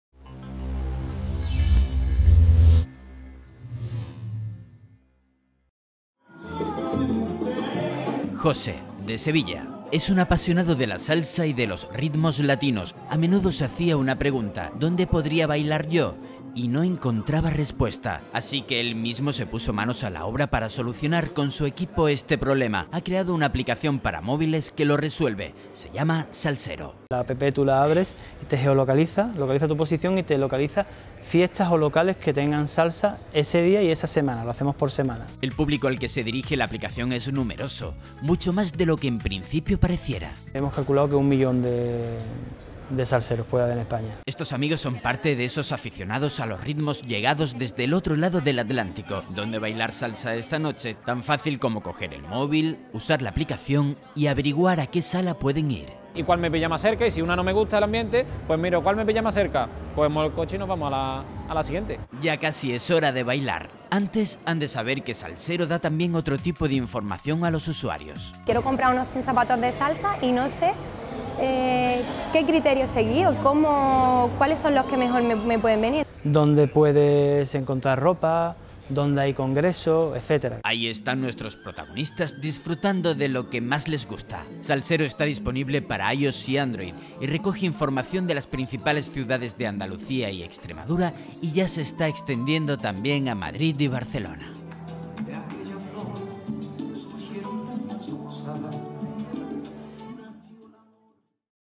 HDL Salsero, una app que saca a bailar a aficionados a la salsa en su ciudad
Jose Crespo es un aficionado a la salsa de Sevilla que habitualmente se hacía una pregunta: ¿Dónde puedo bailar? Y no encontraba respuesta. Por eso, decidió crear junto a su equipo la solución. Es la app Salsero, una herramienta que ayuda al usuario a localizar las salas de baile más cercanas y los eventos que se celebran en ese momento.